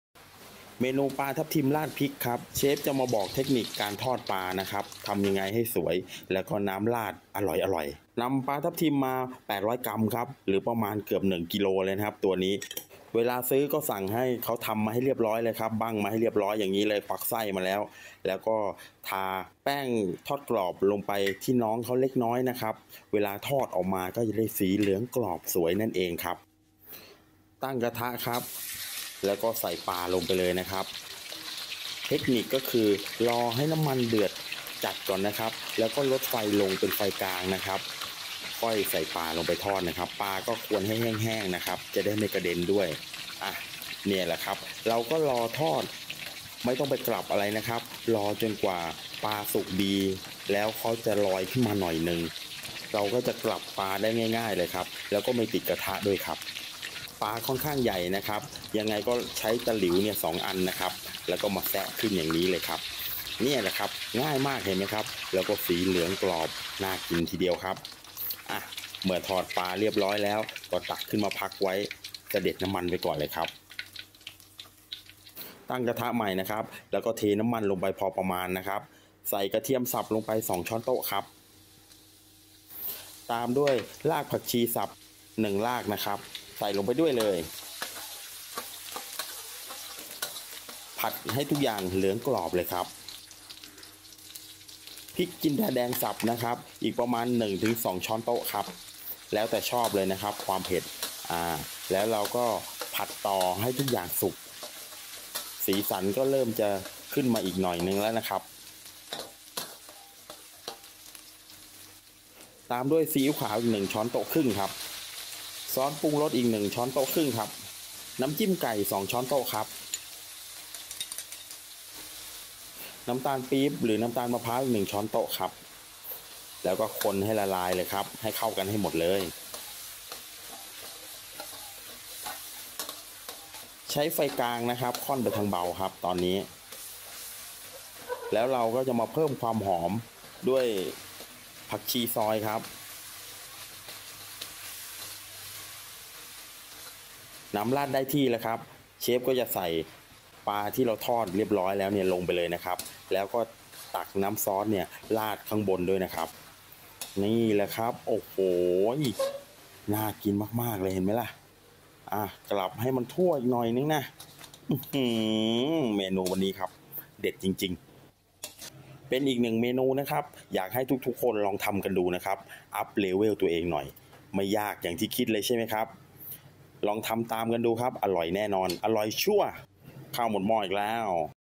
เชฟจอมหอมครัวสอนทำปลาทับทิมราดพริก#ปลาทับทีมราดพริก
เชฟจอมหอมครัวสอนทำปลาทับทิมราดพริก